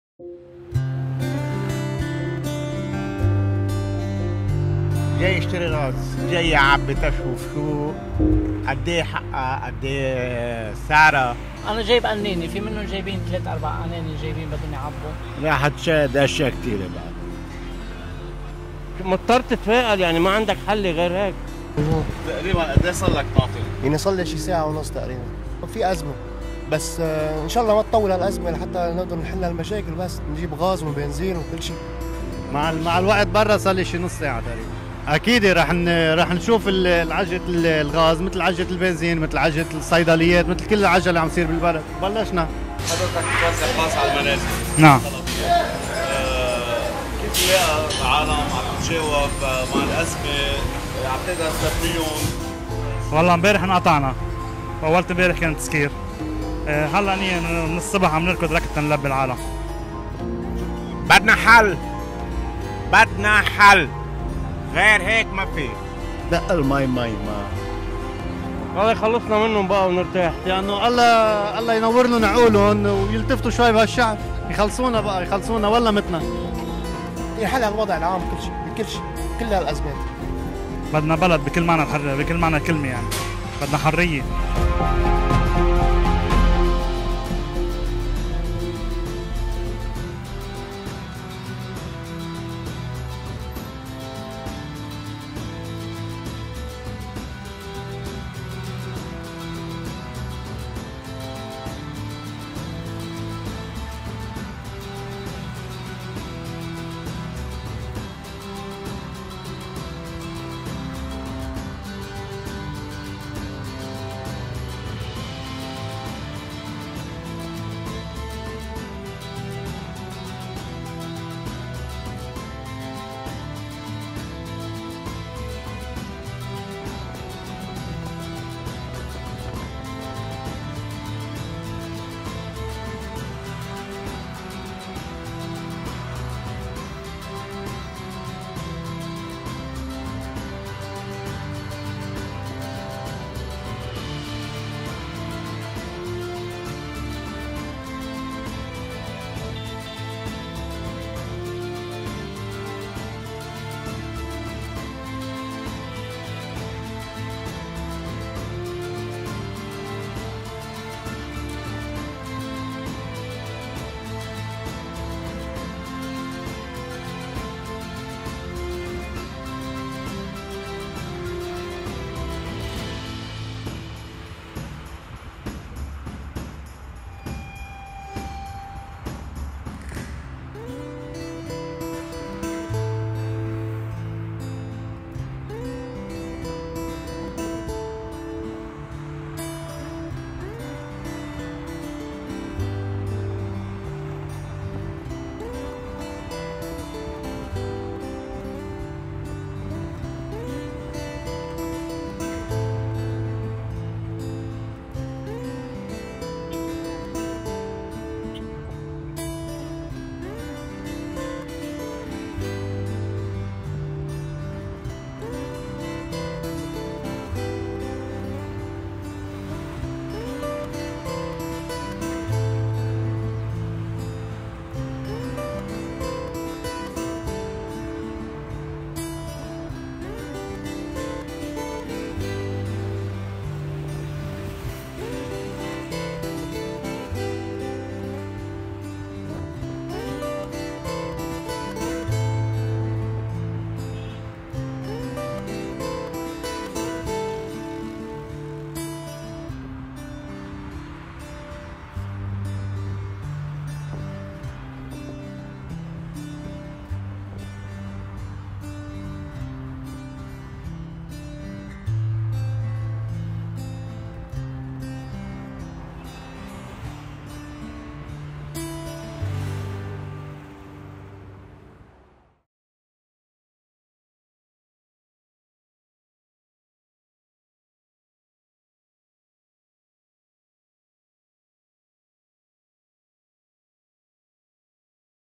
سلسلة ثالثة على خريطة لبنان!
سلسلة جديدة أصبح يمتلكها لبنان، ليست تلك الشرقية أوالغربية لجباله، بل سلسلة ساحليّة حديديّة امتدت بشكلٍ متراص من ساحل جل الديب إلى عمشيت.